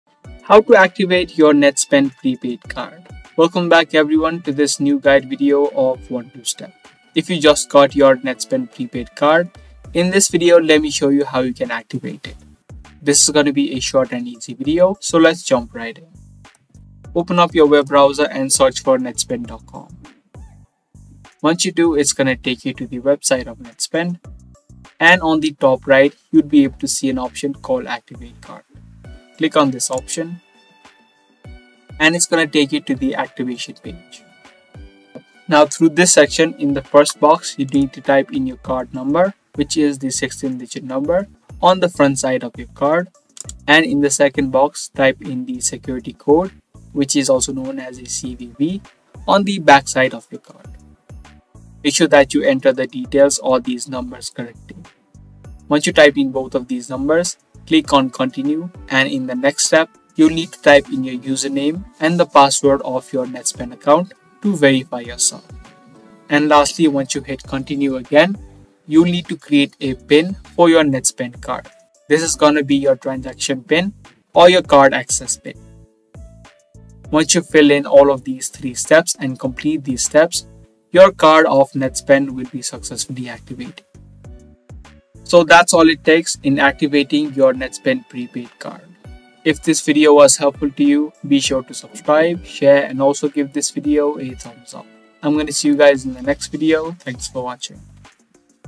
How To Activate Netspend Prepaid Card (2023) | Netspend All Access Card Online
This video walks you through the step by step process of how to activate your netspend prepaid card online. Watch the video till the end and learn the steps in detail on how to activate netspend prepaid visa debit card in 2023.